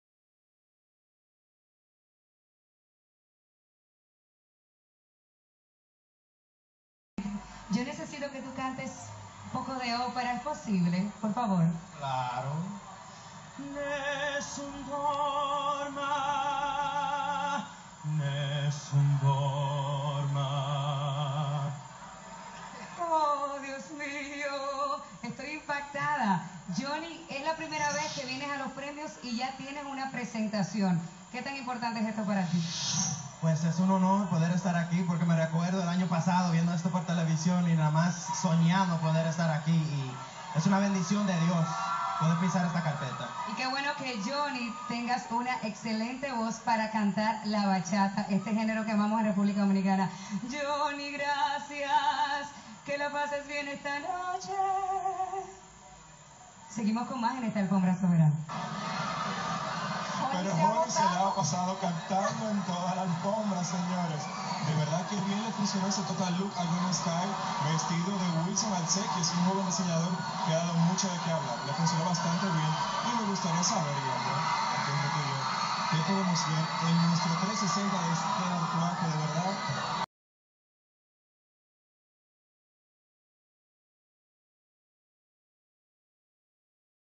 JOHNNY SKY
JOHNNY SKY EN LA ALFOMBRA ROJA DE LOS SOBERANO 2014